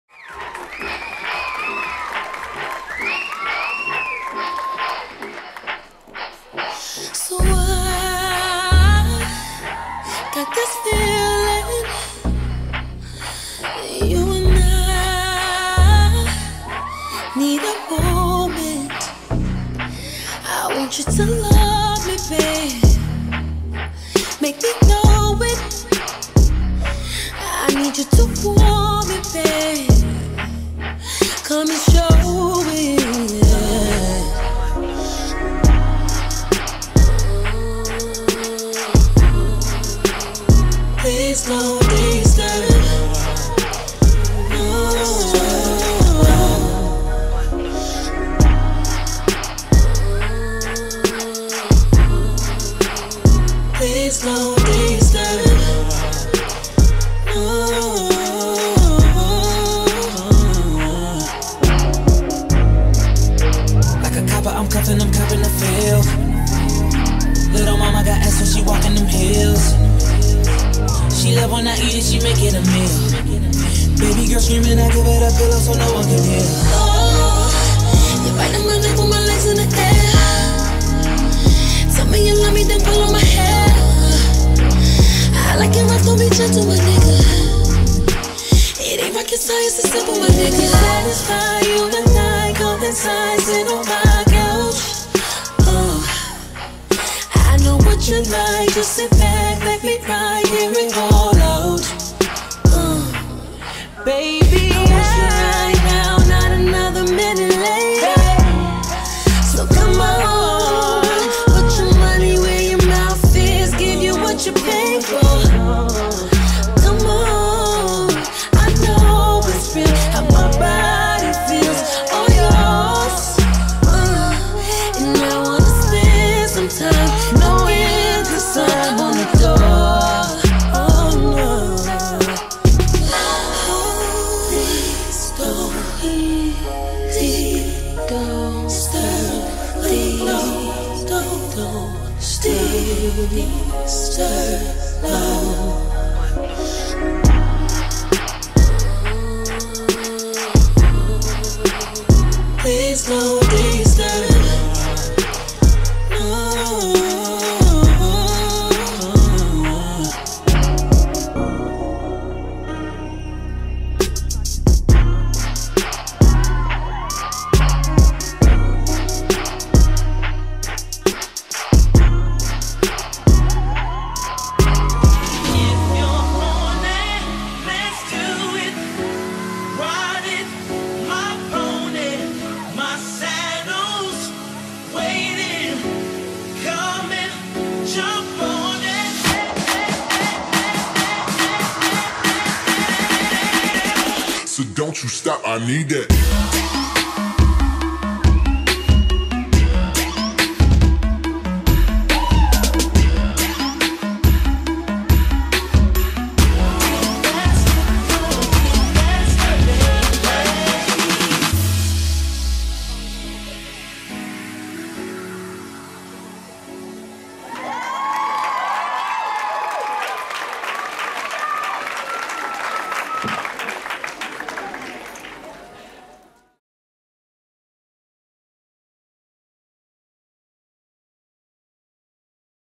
FRISK 2017 - Ménage à Trois: Part I
On Saturday 21st October, 2017, 30 sexy finalists were given the opportunity to express their version of "FRISK". Our audience were in for one wild night!!!

Our opening show featured the ever so amazing Amy Hazel, Gracie Buntine and Taylor Diamond Lord!